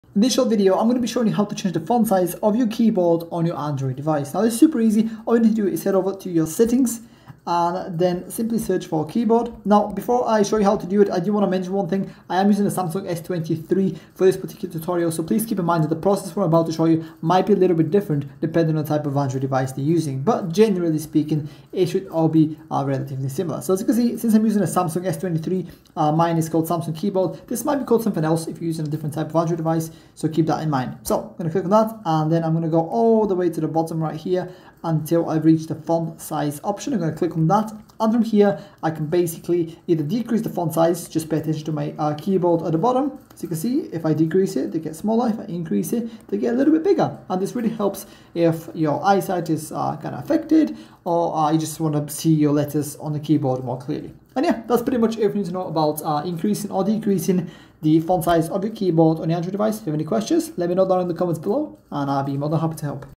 How to Change Font Size of Keyboard on Android
In this short video, I'll show you how to change the font size of your keyboard on Android.